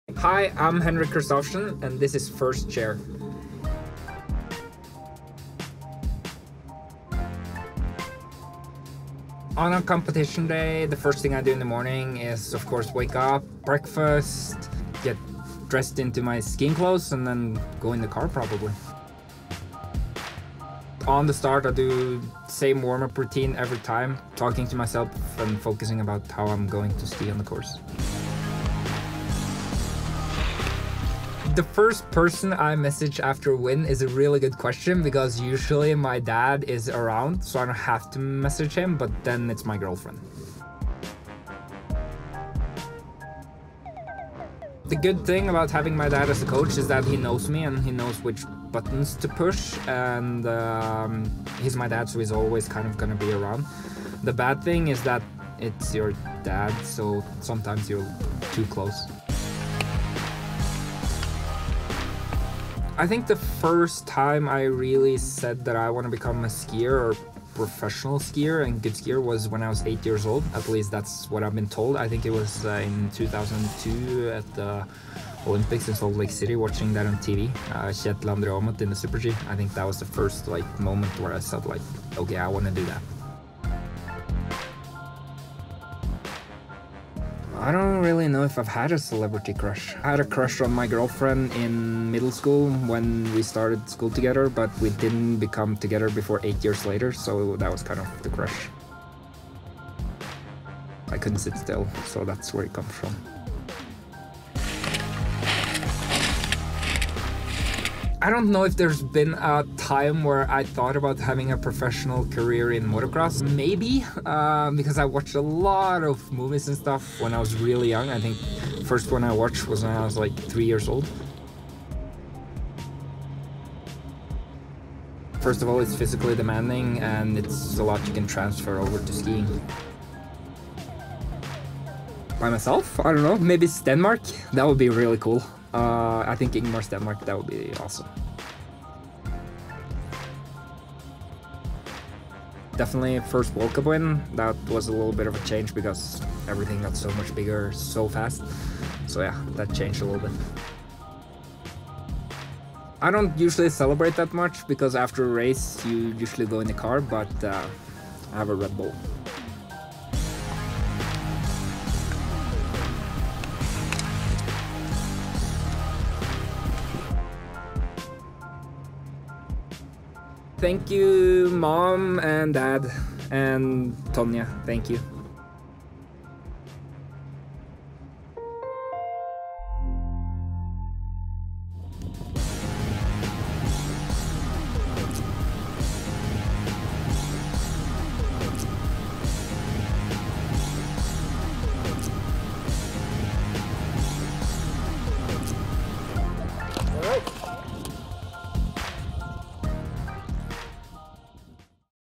Taking the First Chair with Henrik Kristoffersen | First Chair E3
In today's episode, we chat with Henrik Kristoffersen, Norwegian alpine ski racer. See what's behind this sport and what it means to be a pro skier.